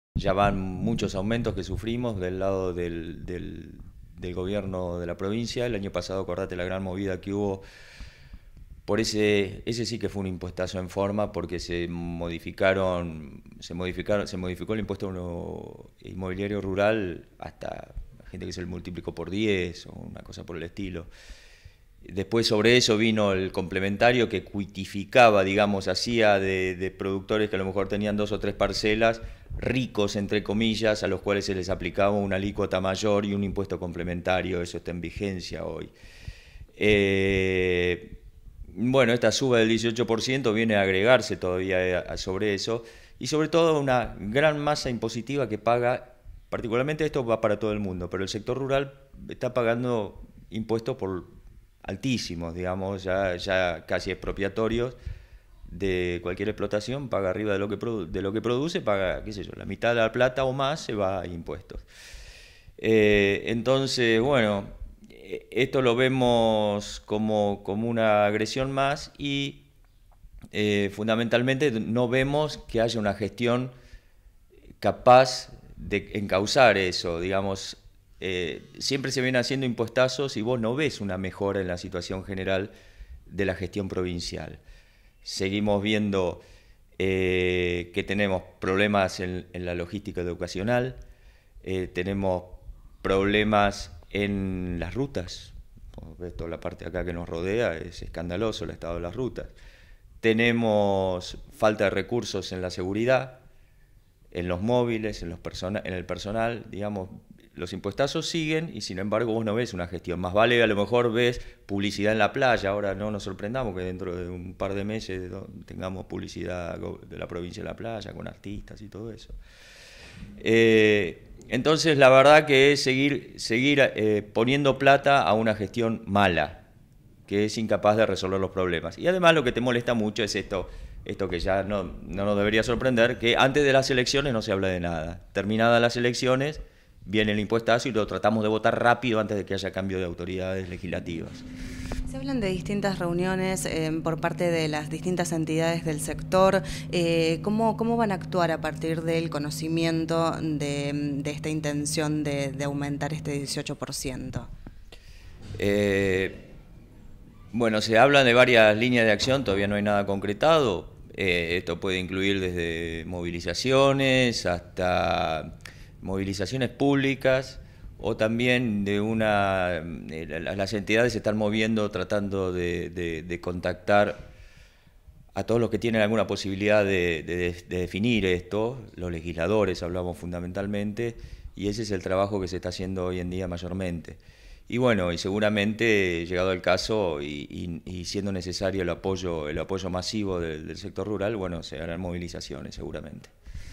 Aumento del Impuesto Inmobiliario:"Los impuestazos siguen y sin embargo vos no ves una gestión"
Ante una eventual suba del impuesto inmobiliario en la provincia, desde el sector rural salieron a desacreditar estas medidas y no descartan movilizaciones. Fernando Luis indicó que no suman, porque no hay mejoras en la gestión.